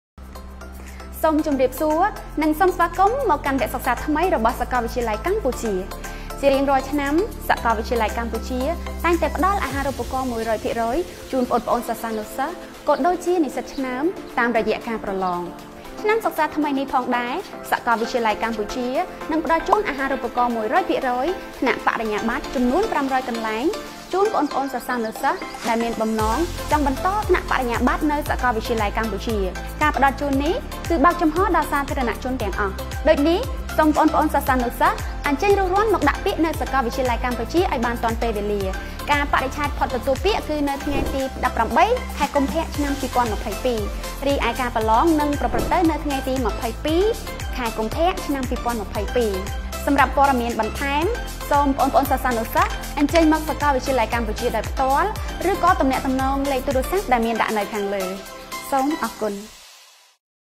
Announcement The University of Cambodia (UC) is offering 500 full-tuition (100%) Peace Scholarships
✍️ចាប់ផ្ដើមទទួលពាក្យពីថ្ងៃផ្សាយដំណឹងនេះតទៅ!
សម្រាប់ព័ត៌មានបន្ថែមសូមទំនាក់ទំនងលេខទូរស័ព្ទ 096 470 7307, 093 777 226
📢 Announcement
The University of Cambodia (UC) is offering 500 full-tuition (100%) Peace Scholarships for the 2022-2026 period, starting in Term 2 (Academic Year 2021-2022).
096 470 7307, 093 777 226, or visit The University of Cambodia during the working hours.